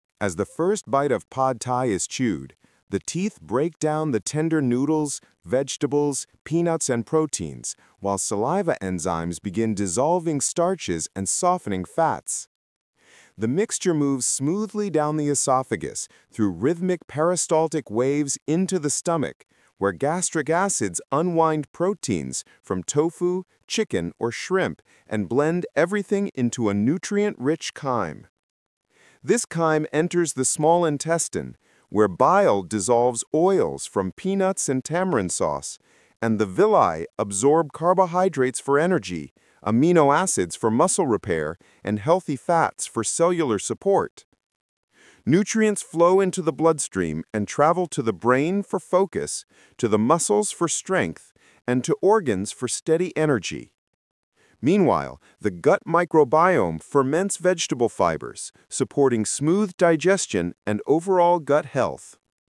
What Pad Thai Does Inside Your Body 🍜
Description:

As the first bite of Pad Thai is chewed, the teeth break down the tender noodles, vegetables, peanuts, and proteins while saliva enzymes begin dissolving starches and softening fats. The swallow carries this flavorful mixture down the esophagus through smooth peristaltic waves until it reaches the stomach, where gastric acids unwind proteins from tofu, chicken, or shrimp and blend everything into a rich, nutrient-filled chyme.

This chyme moves into the small intestine, where bile dissolves oils from peanuts and tamarind sauce, and villi absorb carbohydrates for quick energy, amino acids for muscle repair, and healthy fats for cellular support. Once in the bloodstream, these nutrients travel to the brain for mental focus, to the muscles for strength, and to vital organs to sustain steady daily energy.

Meanwhile, the gut microbiome ferments the vegetable fibers, creating beneficial compounds that support smooth digestion, immunity, and overall gut balance. Pad Thai becomes more than a meal—it becomes a full-body source of nourishment.